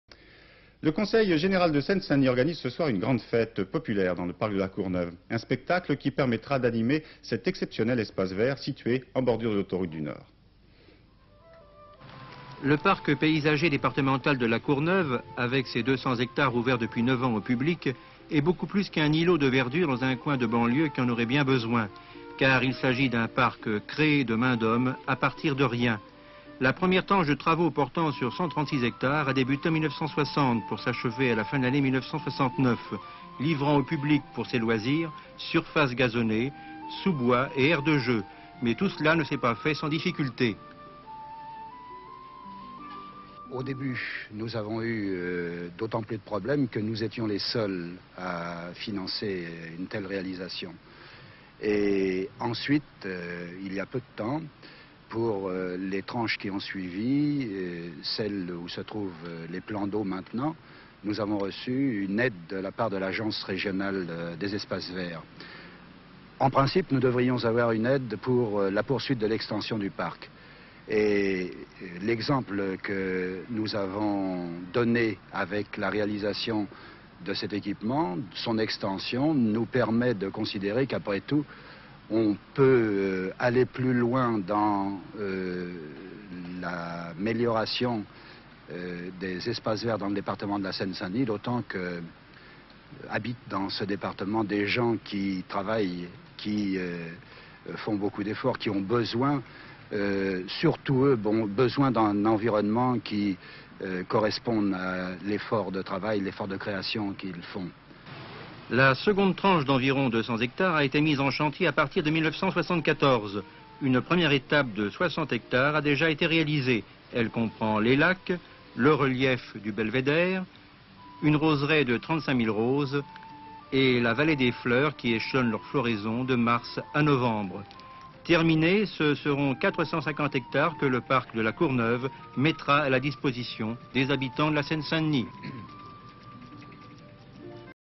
Le parc de La Courneuve
29 juin 1979
Le parc paysager départemental de La Courneuve avec ses 200 ha ouverts depuis 9 ans au public est beaucoup plus qu'un îlot de verdure dans un coin de banlieue, car il s'agit d'un parc créé de main d'homme, à partir de rien. La première tranche de travaux portant sur 136 ha a débuté en 1960 pour s'achever à la fin de l'année 1969.
Interview de Georges VALBON, président du Conseil Général de Seine Saint Denis sur les difficultés financières rencontrées pour créer le parc, les aides obtenues pour son extension.
La seconde tranche d'environ 200 ha a été mise en chantier à partir de 1974. La première étape de 60 ha a déjà été réalisée : elle comprend les lacs, le relief du belvédère, une roseraie de 35 000 roses et la vallée des fleurs qui échelonnent leur floraison de mars à novembre. Terminé, le parc mettra 450 ha à la disposition des habitants de la Seine Saint Denis. Images d'archive INA